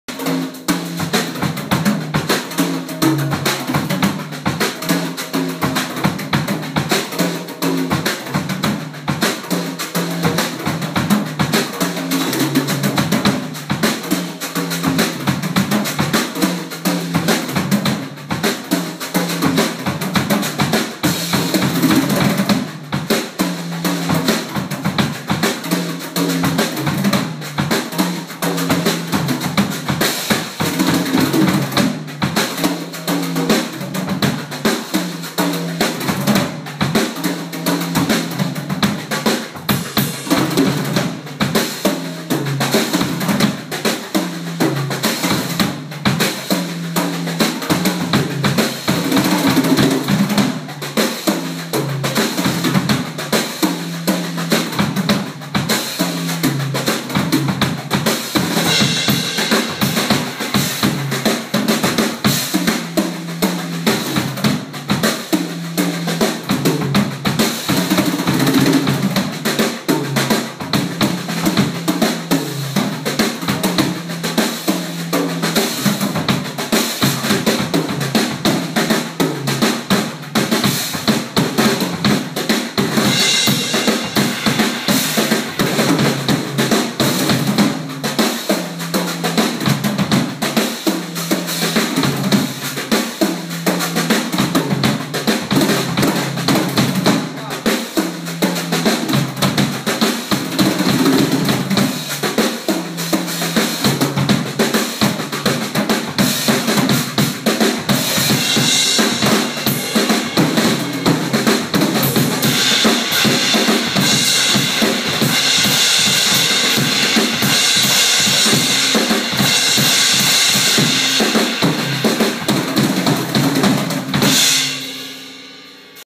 Rick Jordan Drums For Trayvon Martin
Known as one of the worlds greatest drummers in R&B, hip hop/rock, jazz, pop,  and Funk...